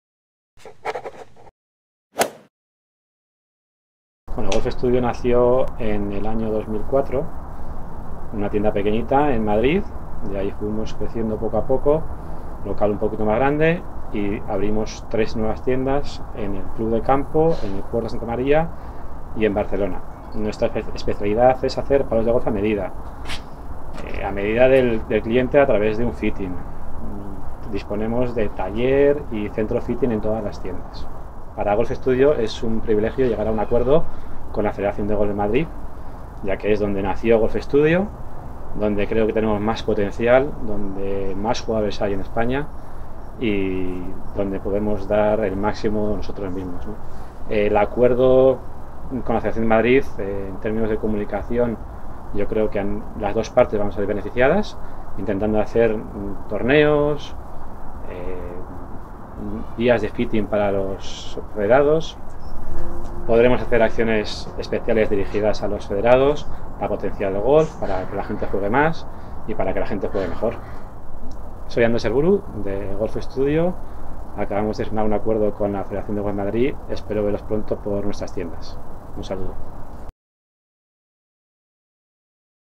Golf Estudio, empresa colaboradora de la FGM para 2020
Andrés Erburu, como representante de Golf Estudio, y la Federación de Golf de Madrid han llegado a un acuerdo por el que Golf Estudio será empresa colaboradora durante el año 2020.